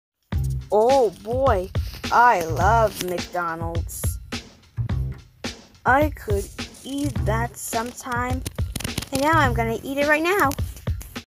My Oh Boy, I Love McDonald's Meme
This Meme is inspired by an episode of Puddle Muddle, where Earl was hugging a jar of honey.